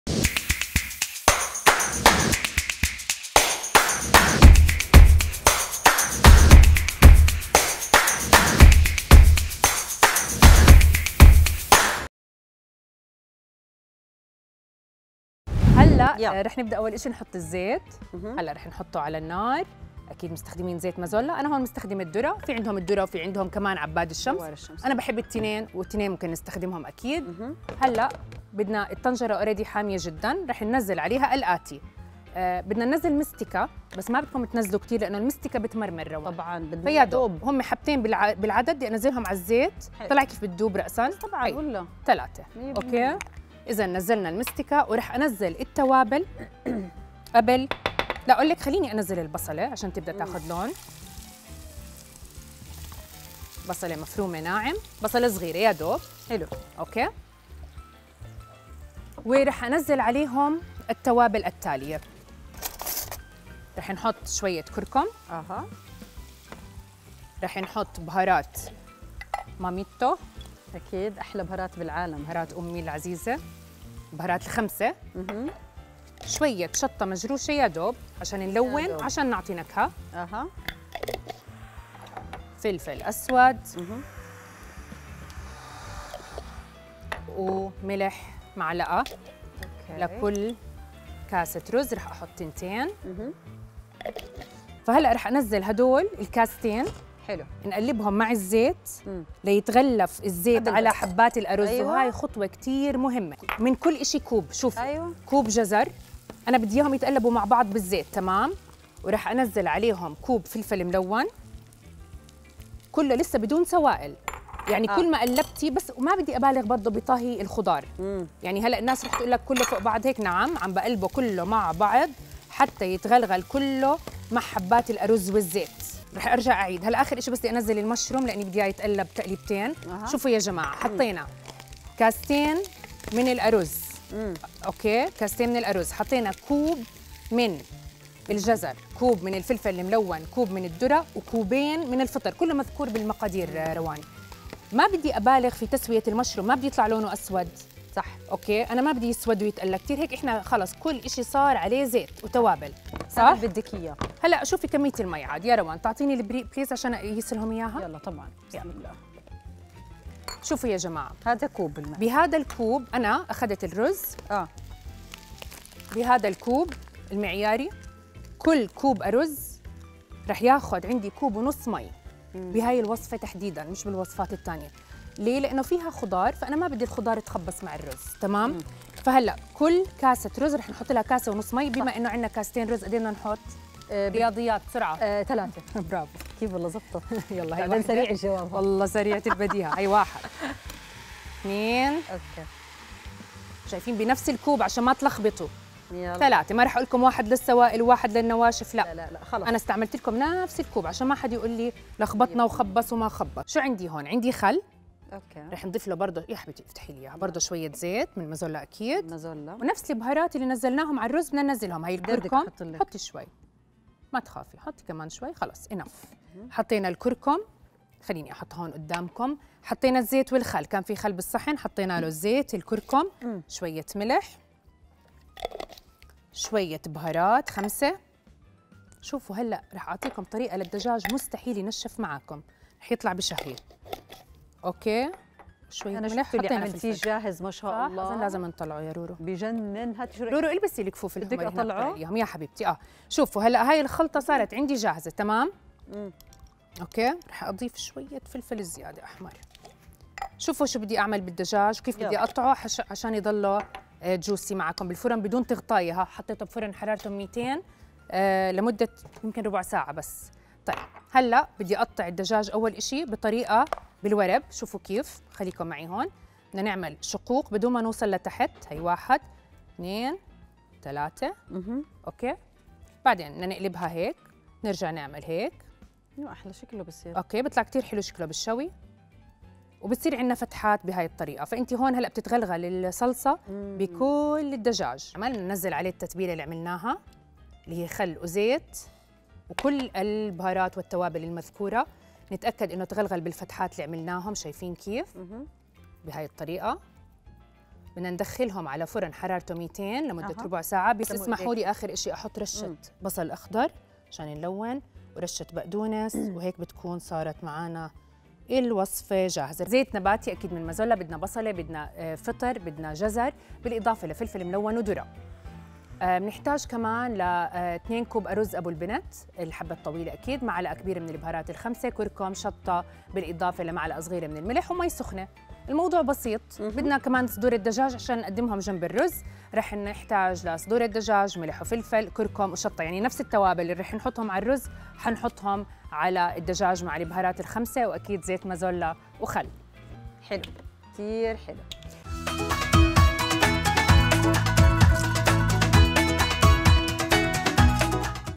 أرز الخضار بالمستكة مع الدجاج المشوي
المقادير :

زيت نباتي مازولا
١ بصلة مفرومة
٢ كوب فطر مفروم
١ كوب جزر
١ كوب فلفل ملون
١ كوب ذرة
٢ كوب أرز أبو البنت حبة طويلة
١ ملعقة كبيرة بهارات خمسة
ملعقة صغيرة كركم
ملعقة صغيرة شطة مجروشة
ملح
ماء ساخن
للدجاج :
٦ صدورة دجاج
ملح وفلفل
ملعقة كبيرة كركم
ملعقة كبيرة ٥ بهارات
٤/١ كوب زيت نباتي مازولا
٤/١ كوب خل

مطبخ رؤيا